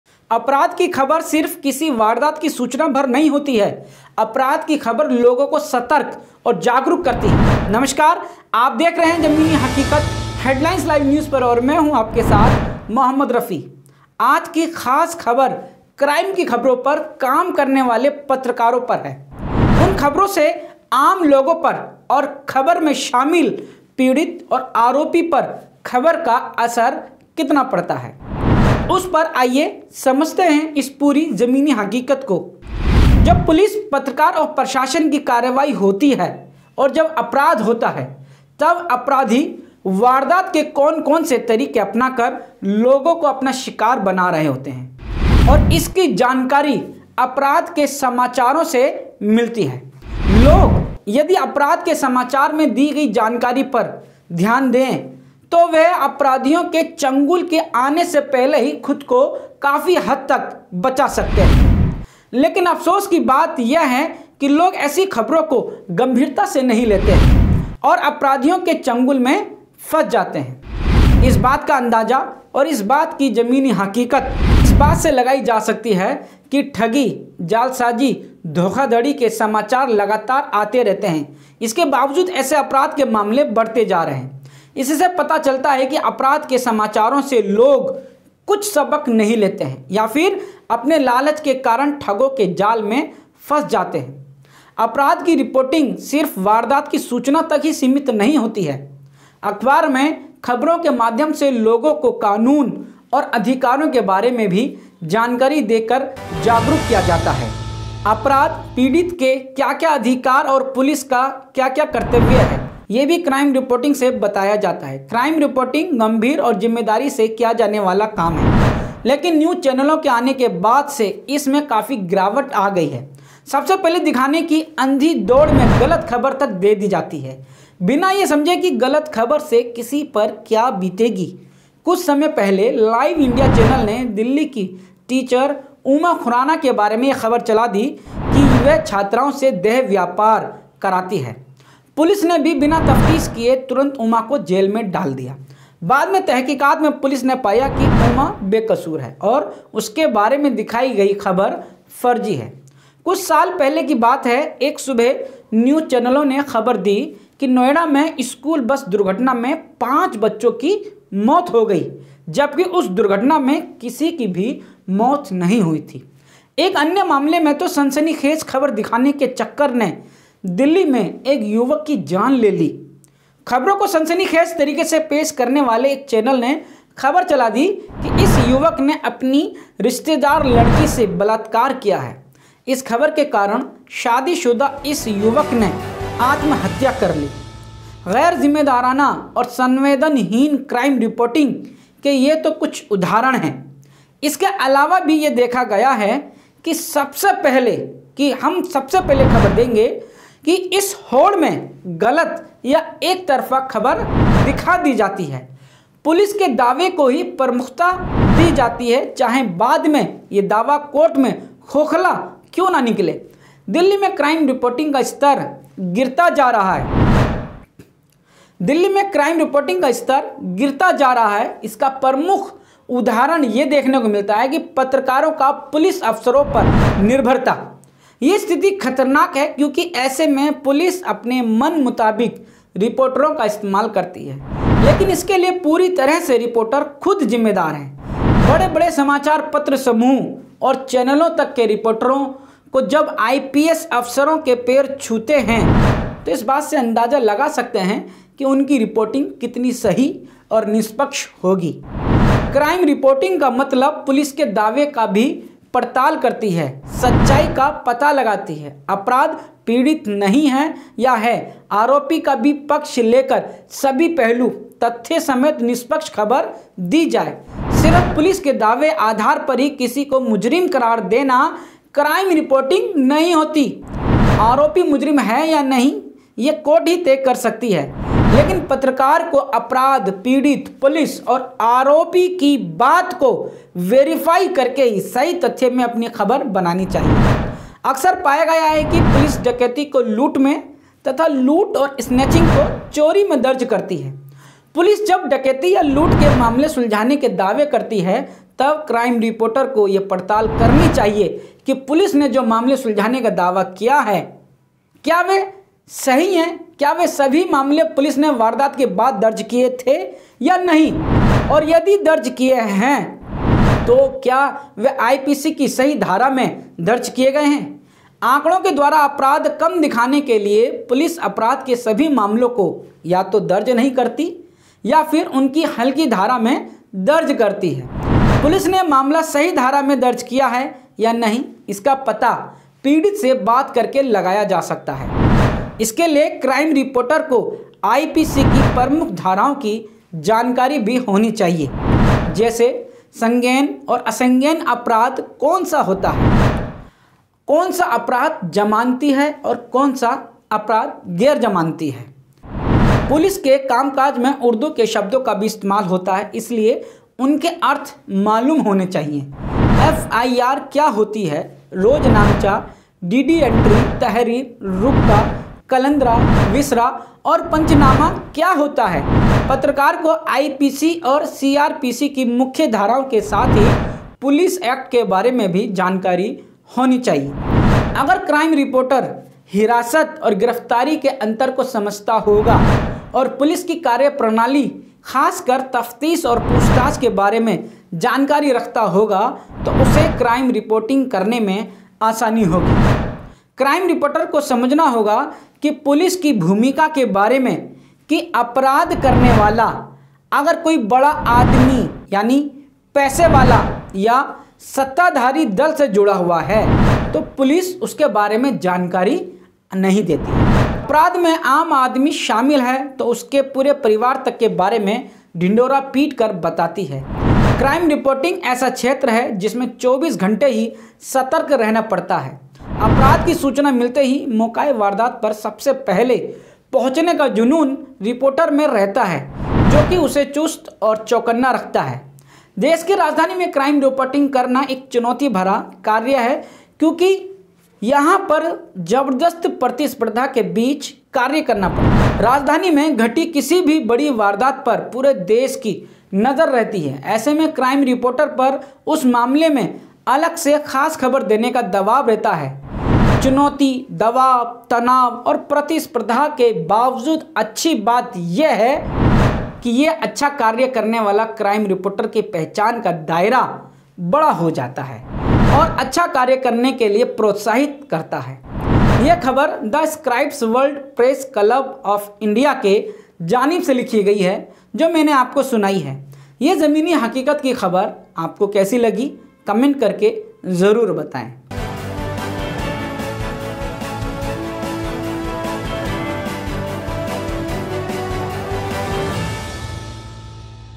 crime reporter और police की जुगल बंदी
News about : क्राइम रिपोर्टर और पुलिस की जुगल बंदी | Crime reporter | police | Headlines Live News

क्राइम रिपोर्टर और पुलिस की जुगल बंदी
क्राइम रिपोर्टर को मालूम होना चाहिए
जल्दबाजी की होड़ में खबर का असर उल्टा पड़ता है

                   t=Cm3bqMFrCVMOAN6Dxbgwrg&s=09

                       i=flaiy8s0qgc7&utm_content=4435sax

Headlines Live News , jamini hakikat